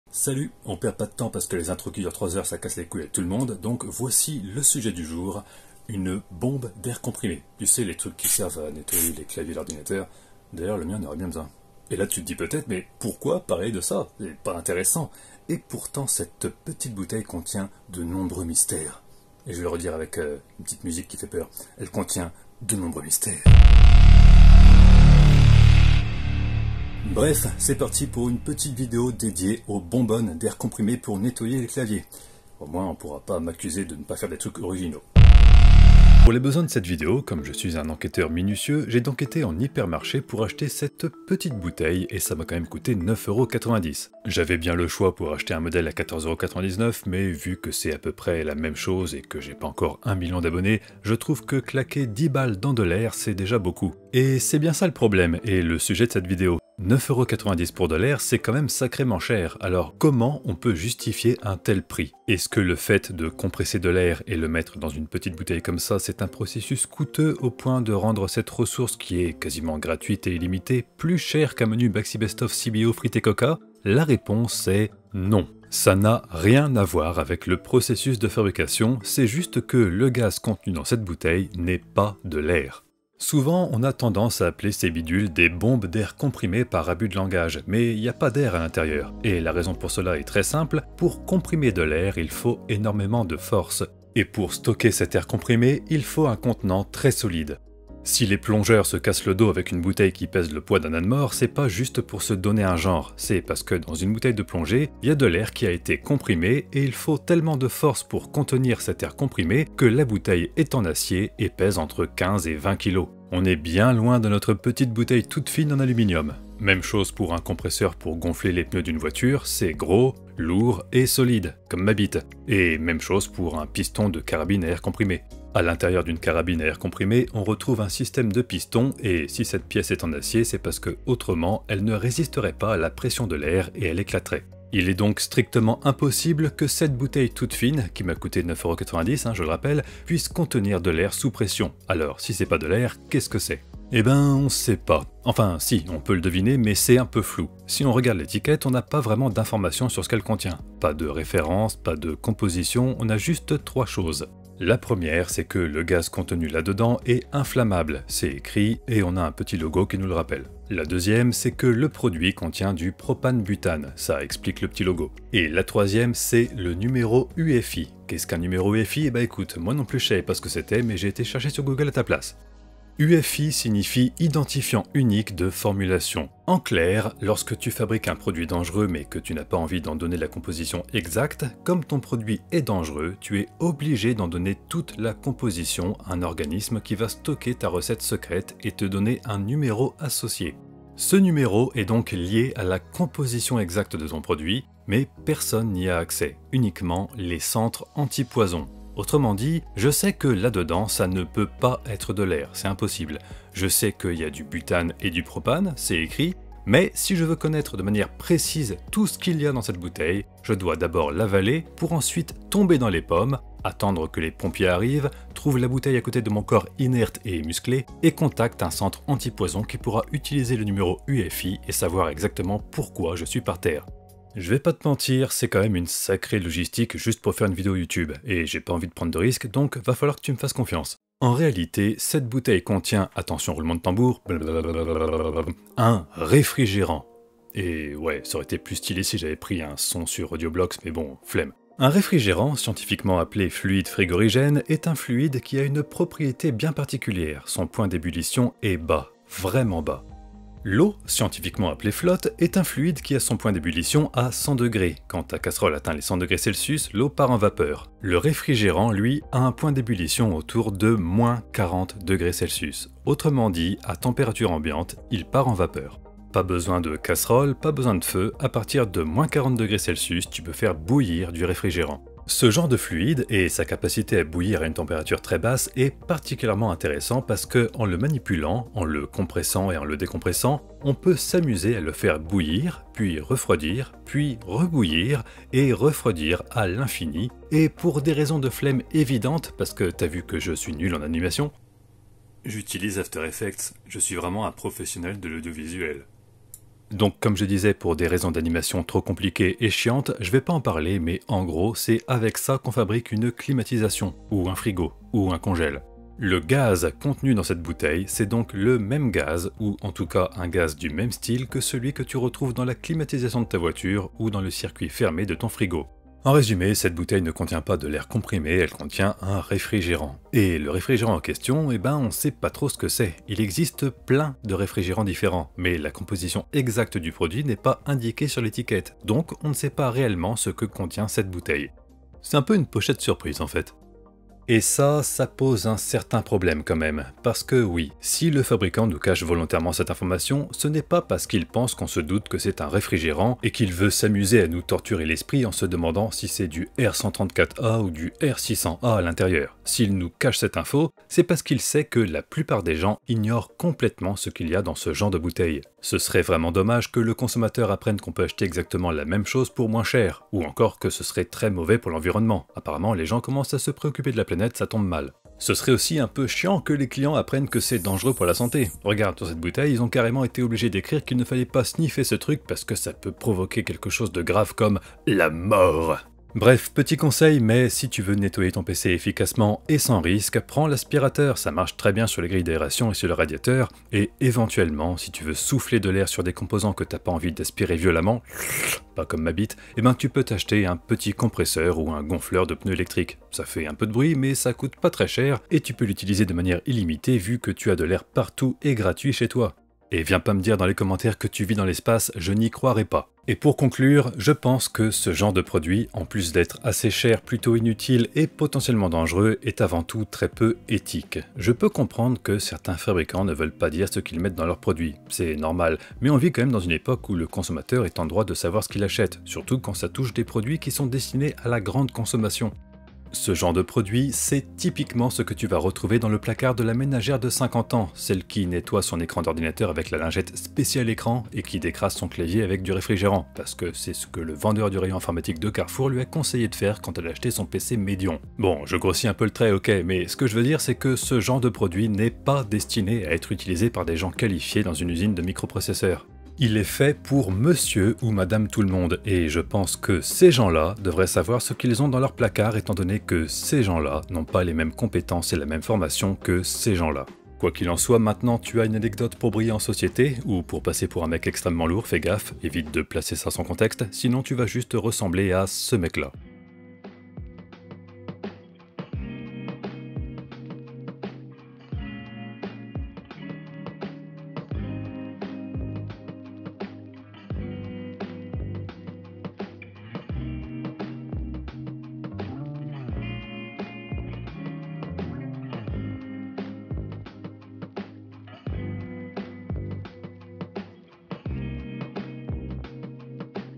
Vidéo dédiée aux bouteilles d'air comprimé pour nettoyer les claviers. (Il en fallait bien une.)
Je parie que tout le monde s'en fout.